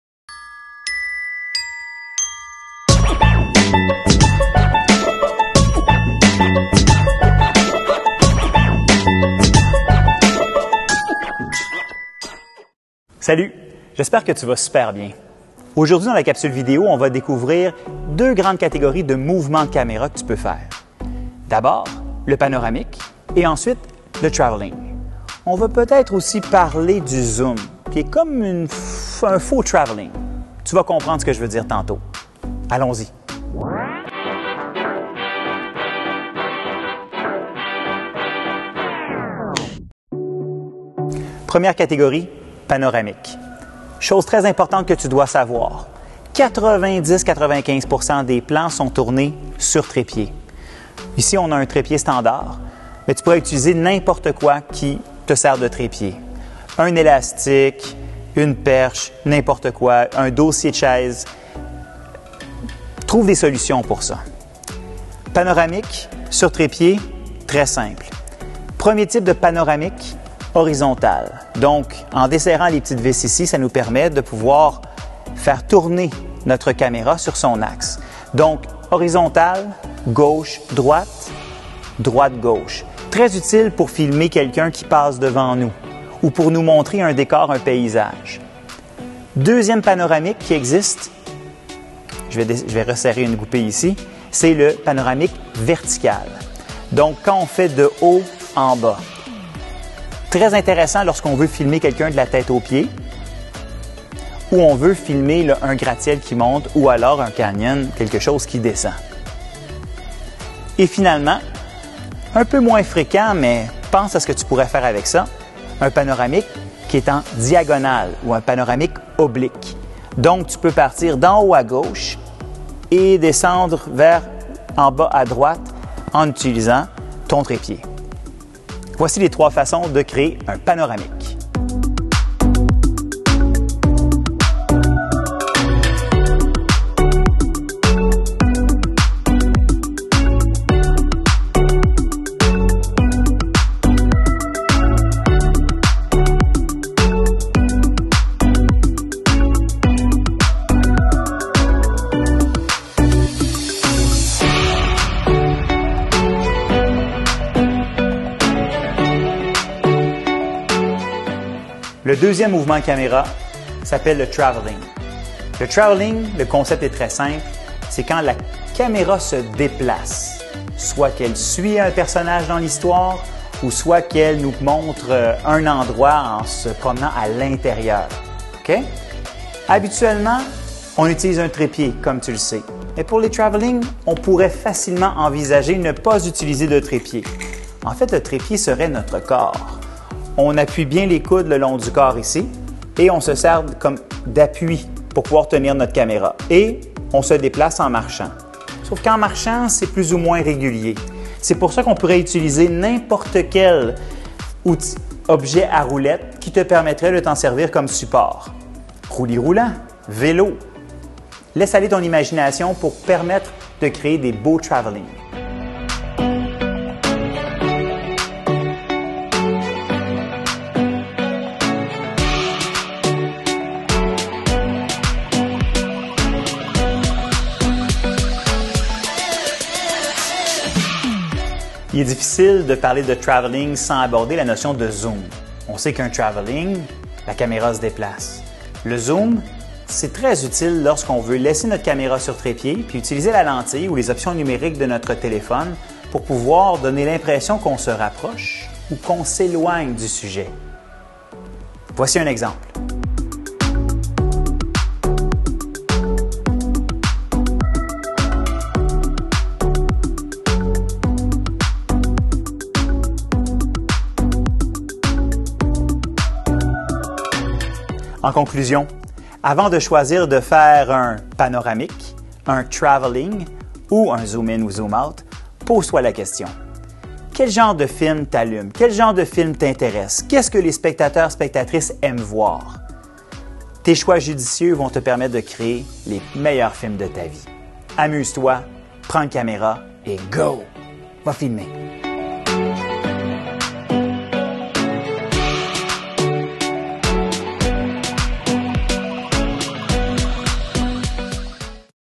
MOUVEMENTS DE CAMÉRA
Voici une capsule explicative concernent le langage multimédia : différents mouvements de caméra lors d'un tournage.
Capsule didactique de type classe inversée a été réalisée pour le cours de formation à distance (FAD) de 4e secondaire en arts plastiques